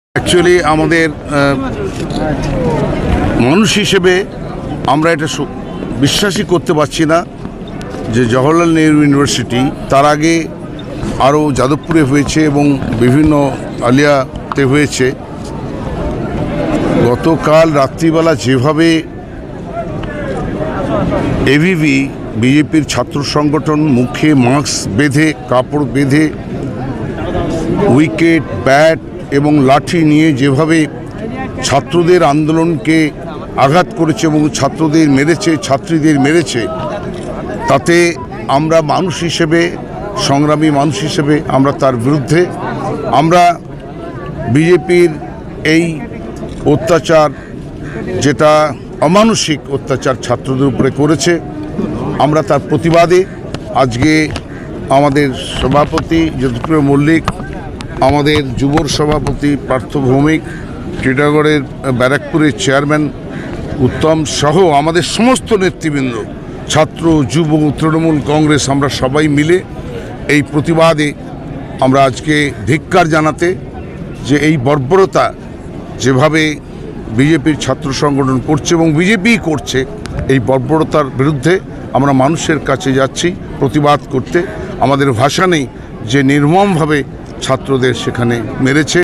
বিজেপি যেভাবে বর্বরোচিতভাবে ছাত্রদের ওপর হামলা চালিয়েছে তার ধিক্কার জানানোর কোনো ভাষা নেই:নির্মলঘোষ
বিজেপি যেভাবে বর্বরোচিতভাবে ছাত্রদের ওপর হামলা চালিয়েছে তার ধিক্কার জানানোর কোনো ভাষা নেই বলে জানালেন নির্মল ঘোষ